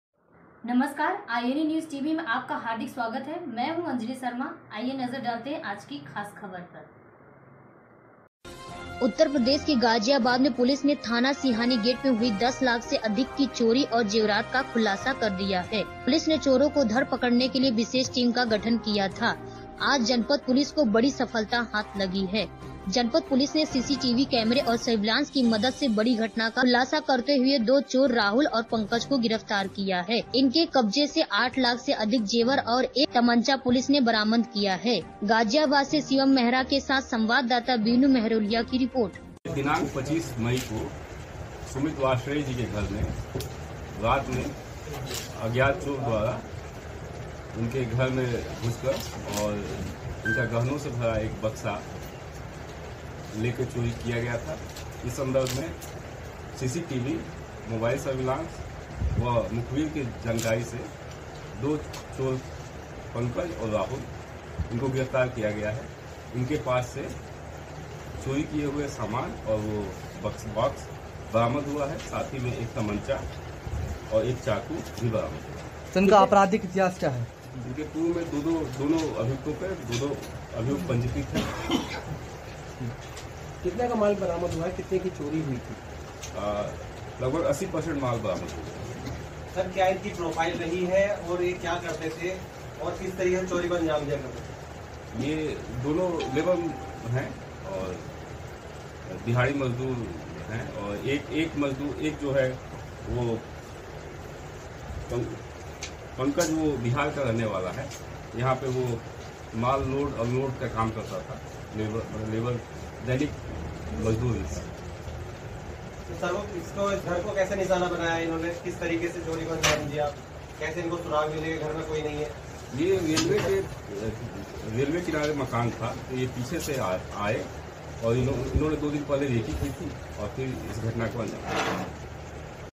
गाजियाबाद : 10 लाख की चोरी का पुलिस ने किया खुलासा | INA NEWS TV || INA EXPRESS
ani news
ina photos

CMD/Editor-in-chief : Shri Uday Singh Yadav